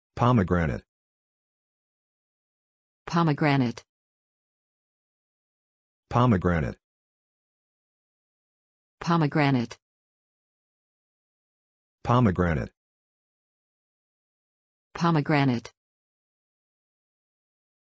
How to Pronounce "Pomegranate"
Learn how to say "Pomegranate" with the correct pronunciation.